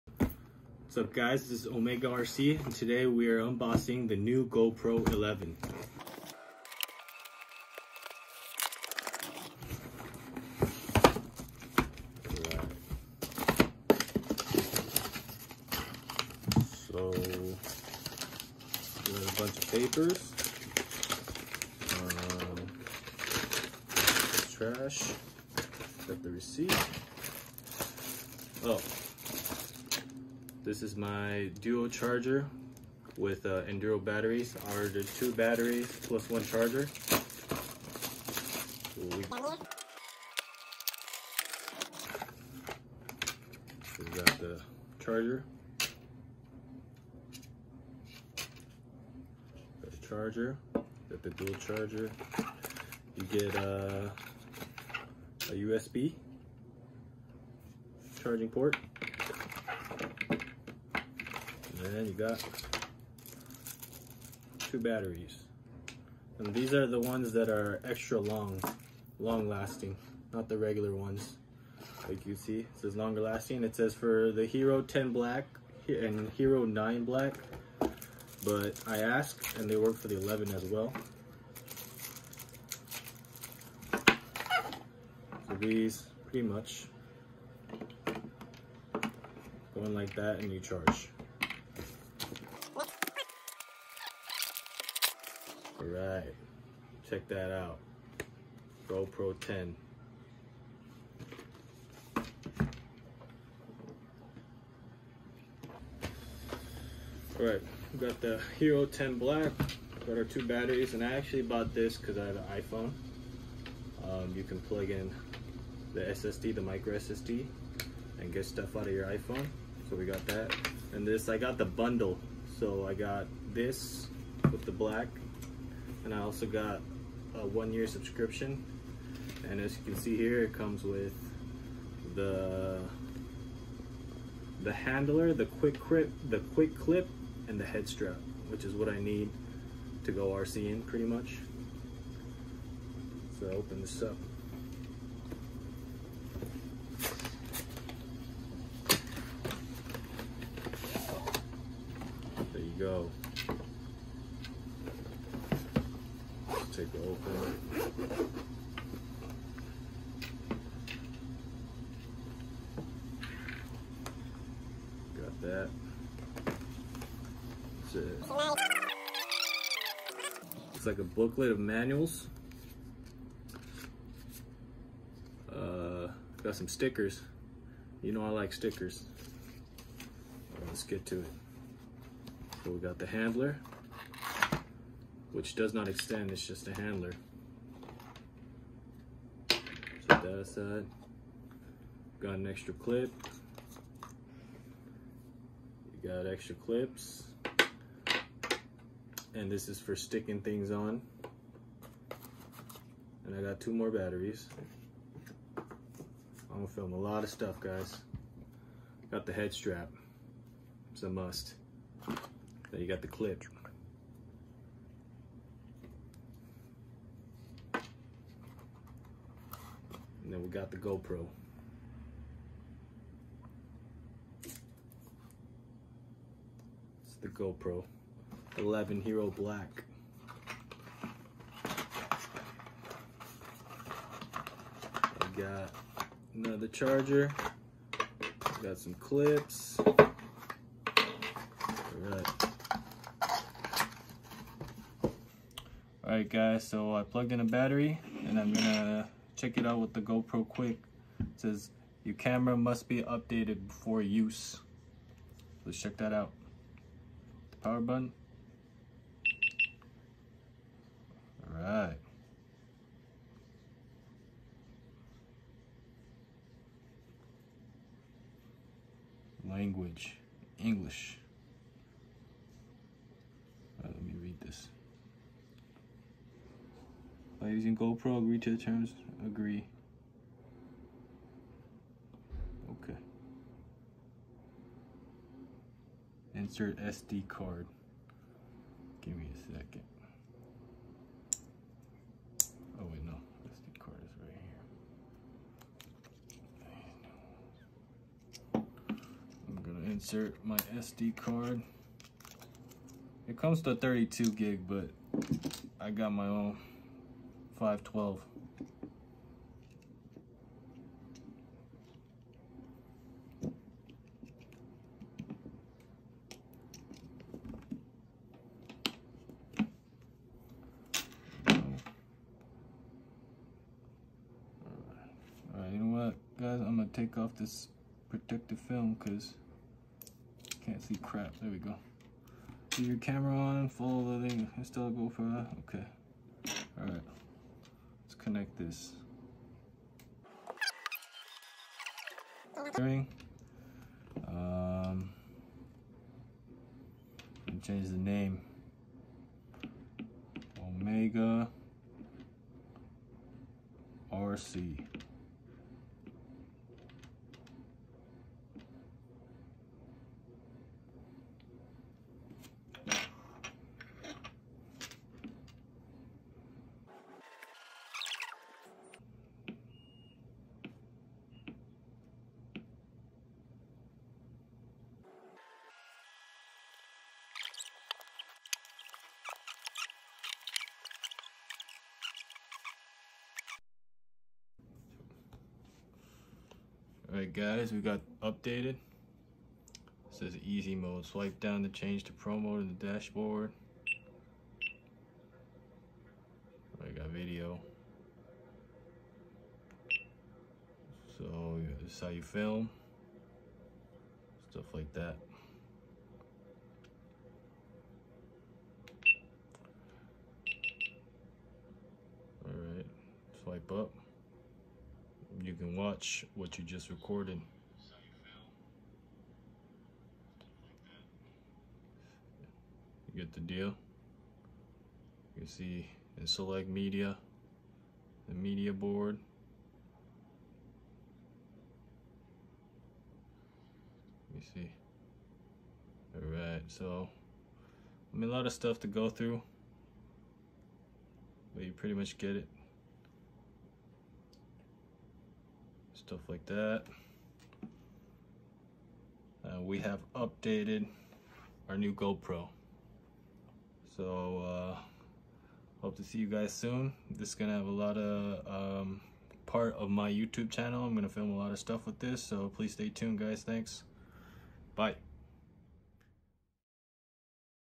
UNBOXING THE NEW GOPRO HERO 11 BLACK!!!
First video ever on the new channel and what better way to start it by unboxing the camera that will record most of the footage.